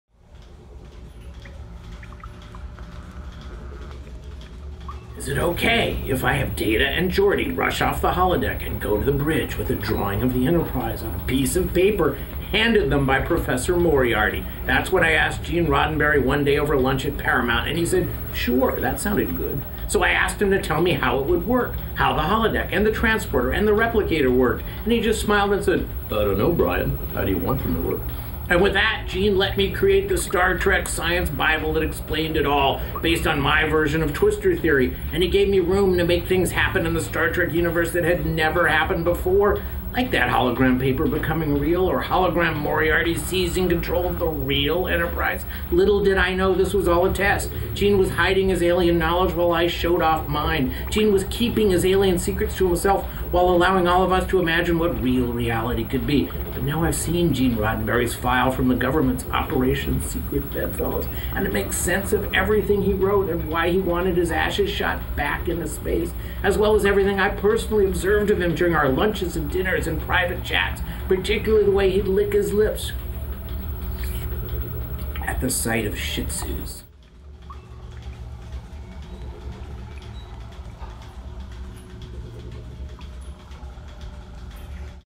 GENE RODDENBERRY EATS SHIH TZUS - SCI-FI STAR TREK TNG
BRIAN LANE DISCUSSES WORKING ON STAR TREK AND MEETING WITH GENE RODDENBERY. EDGY SCI-FI COMEDY SERIES "SECRET BEDFELLOWS", FROM WRITER BRIAN LANE AUTHOR OF STAR TREK TNG'S FAN FAV EPISODE "ELEMENTARY, DEAR DATA". SEE THE PILOT EPISODE. THERE ARE NO EARTHLINGS!